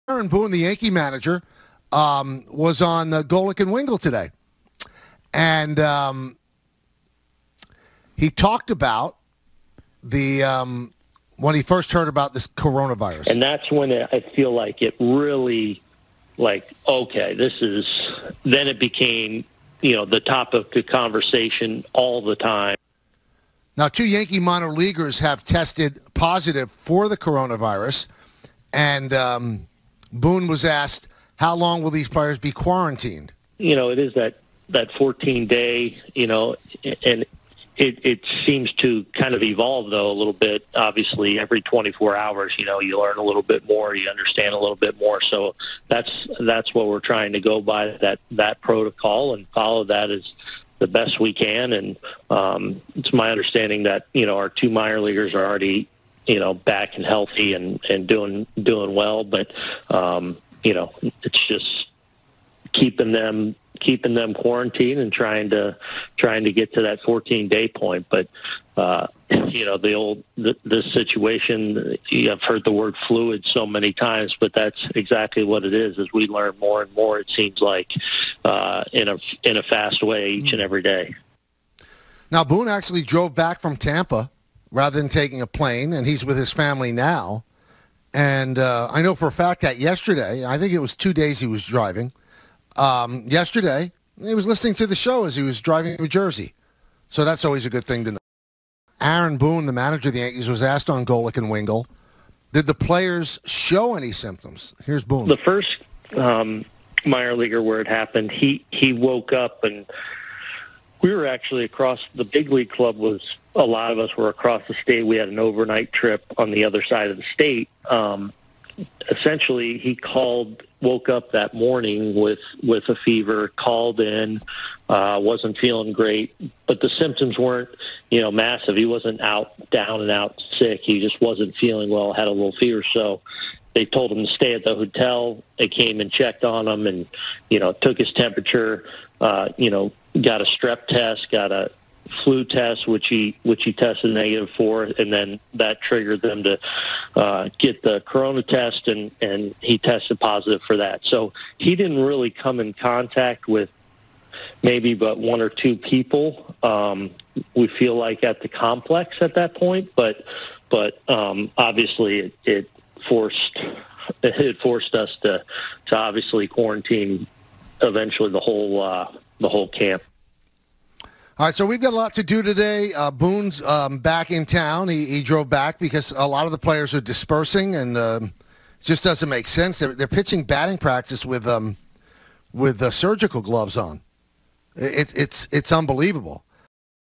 Aaron Boone on the latest Yankees COVID-19 developments
Aaron Boone's reaction to COVID-19 virus news within the Yankees organization.